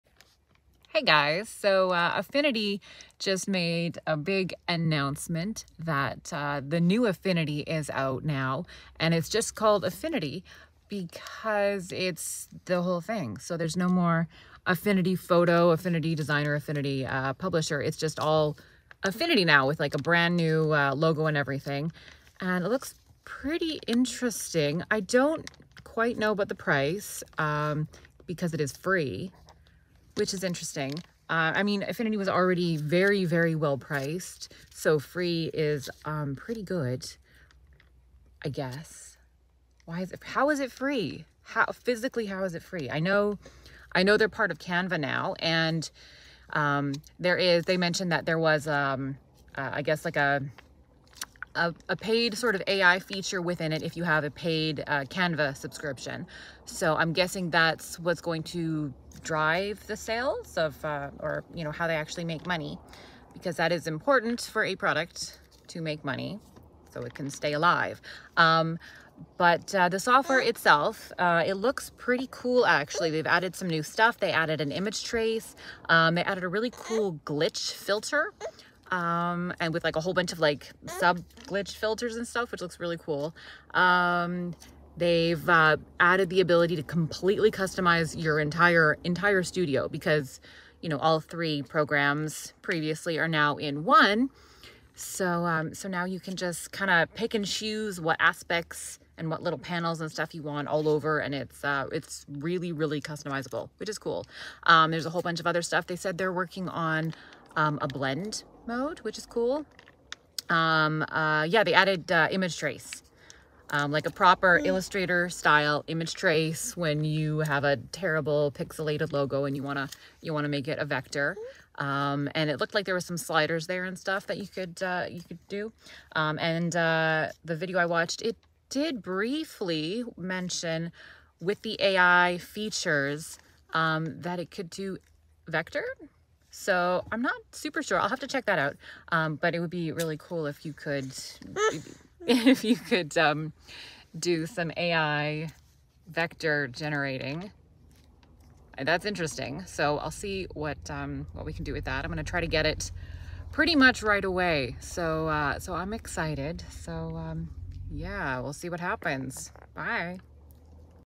A Brand New Affinity? Complete Overhaul of the old Affinity programs - And it’s free??!!
The Affinity team just unannounced the new version of affinity and it’s all three programs in one, and even more unbelievably, it’s free. They’ve added new features like image trace and a mesh, and a really cool glitch filter. They’ve also paired it with Canva for some AI tools. I can’t wait to check it out!￼